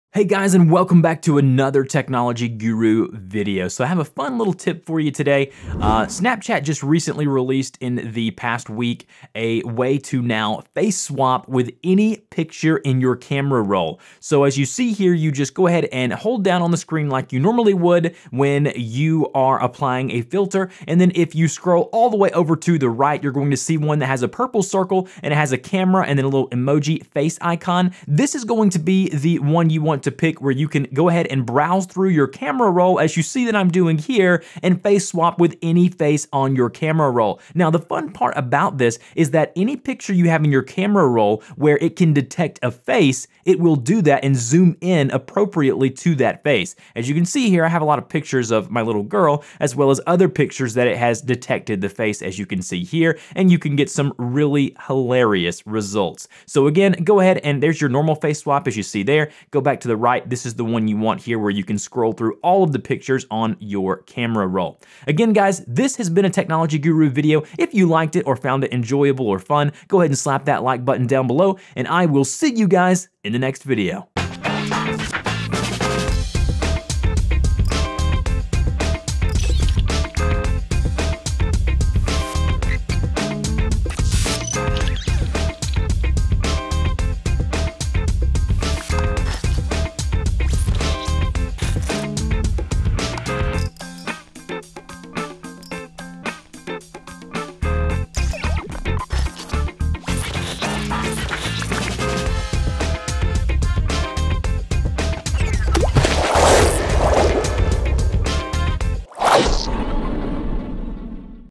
How To Face Swap With Any Picture In Your Camera Roll - Snapchat Tutorial
In this video tutorial, I show you how to easily swap your face with any picture in your camera roll! YOU MUST HAVE THE MOST RECENT VERSION OF SNAPCHAT FOR THIS TO WORK.

If you have any questions feel free to put them in the comment box below OR ask them on our community forums here:

dmporter74